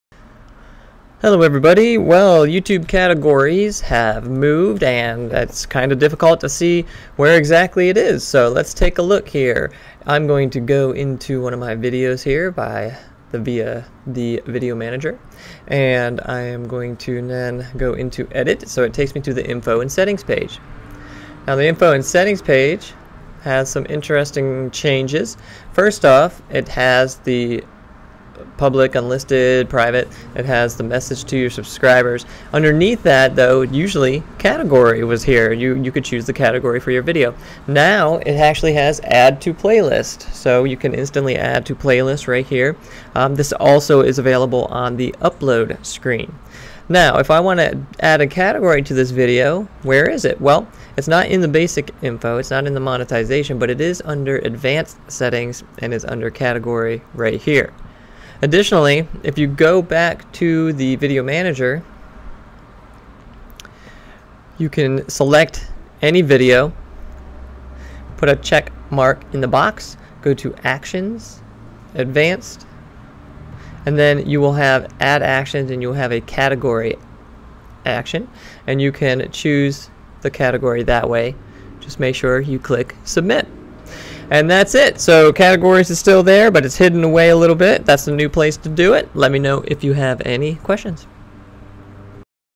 YouTube Categories Have Moved
YouTube categories are now missing from the Info and Settings page as well as the Upload page. So where can you still access them? Find out here.

• YouTube Marketing
• Video Marketing
• Online Business
• eCommerce